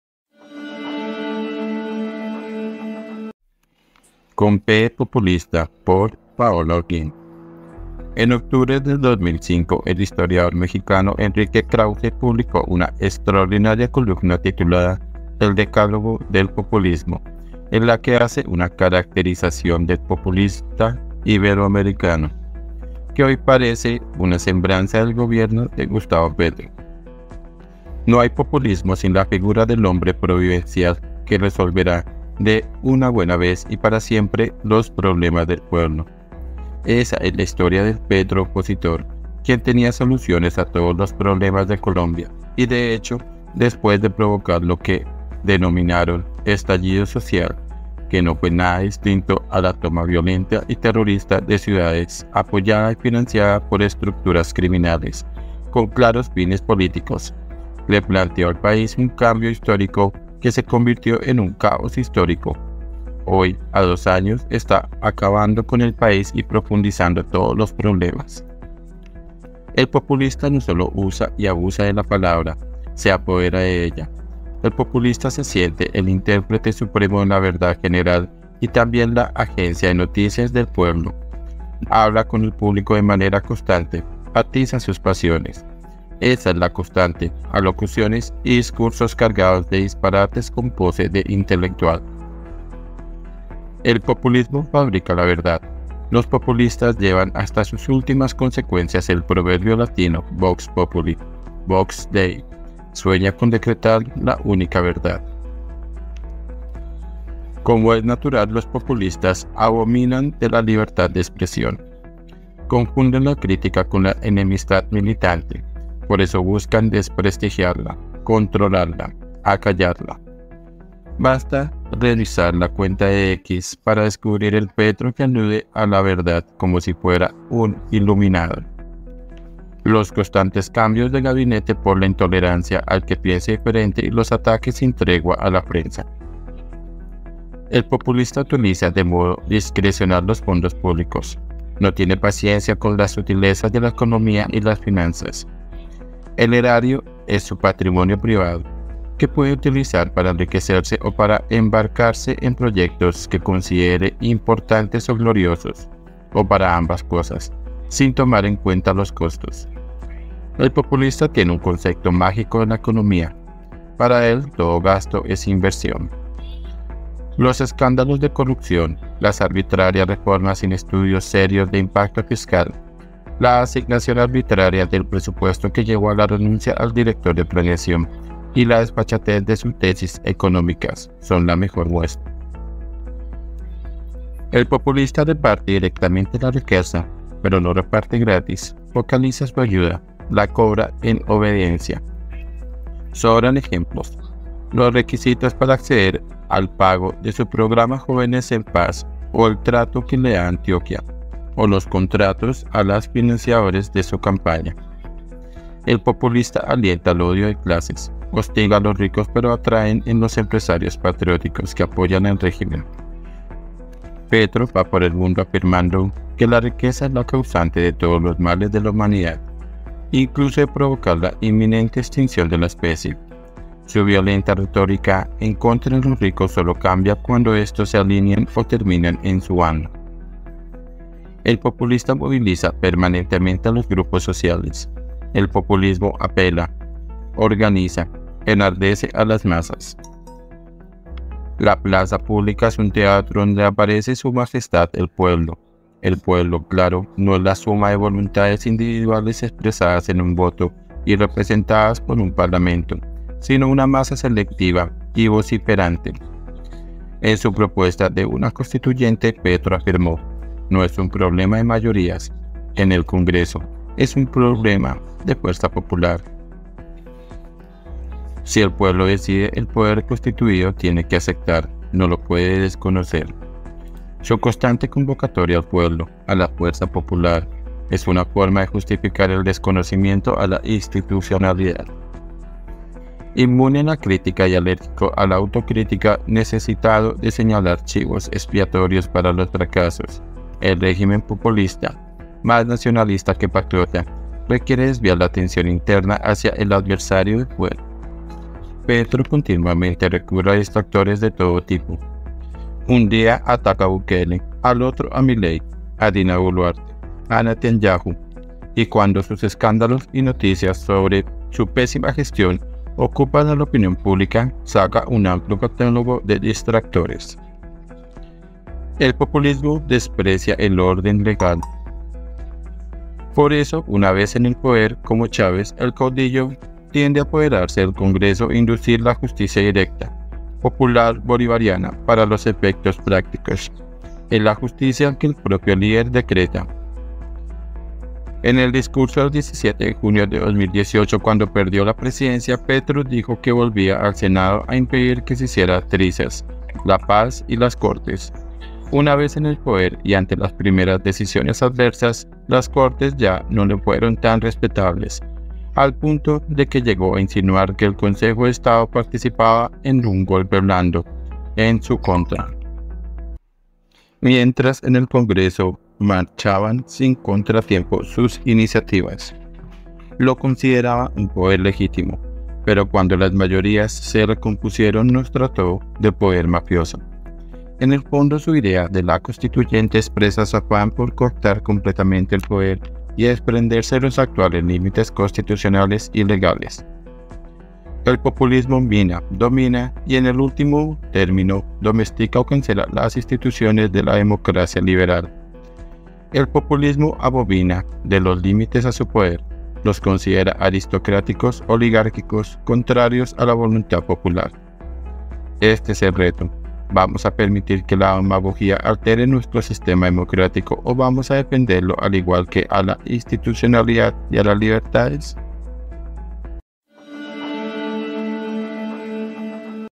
CON "P" DE POPULISTA ⚫ Por: Paola Holguín
En octubre del 2005, el historiador mexicano Enrique Krauze publicó una extraordinaria columna titulada ‘El Decálogo del Populismo’, en la que hace una caracterización del populista iberoamericano, que hoy parece una semblanza del gobierno de Gustavo Petro.
columna PAOLA HOLGUÍN